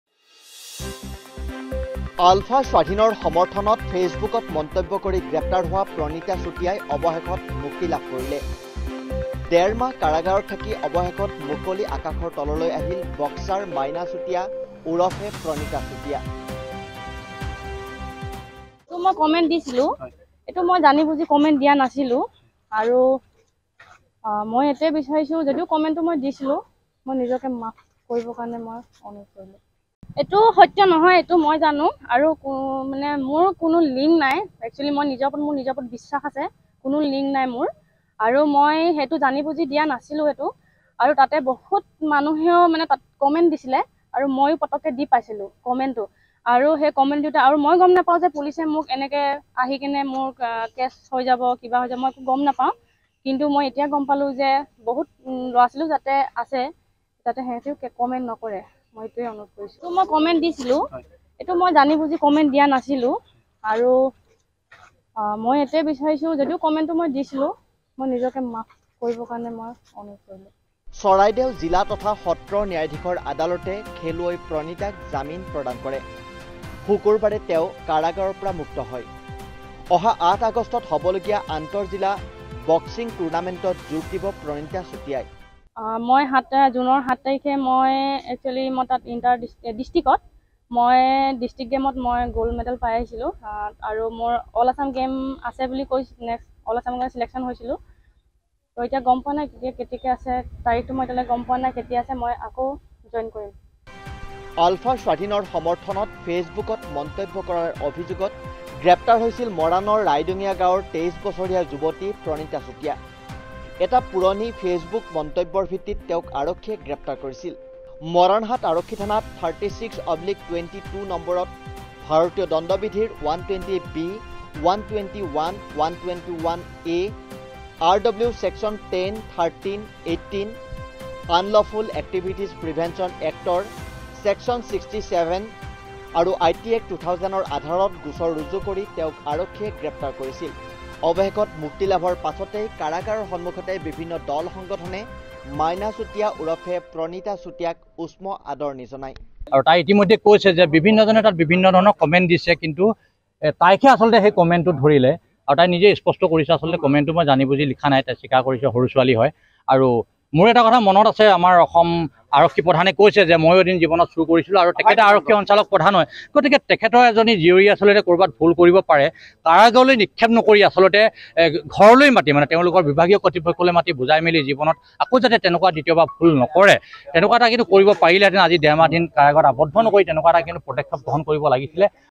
কাৰামুক্ত আলফা(স্বাঃ)ৰ সমৰ্থনত মন্তব্য কৰি জেইললৈ যোৱা বক্সাৰ প্ৰণীতা চুতীয়া
ডেৰ মাহ কাৰাগাৰত থাকি অৱশেষত মুক্ত হ’ল বক্সাৰ প্ৰণীতা চুতীয়া।
চৰাইদেউ জিলা তথা সত্ৰ ন্যায়ধীশৰ আদালতে খেলুৱৈ প্ৰণীতাক জামিন প্ৰদান কৰে।
আলফা(স্বাঃ)ৰ সমৰ্থনত মন্তব্য কৰাৰ অভিযোগত গ্ৰেপ্তাৰ হৈছিল প্ৰণীতা।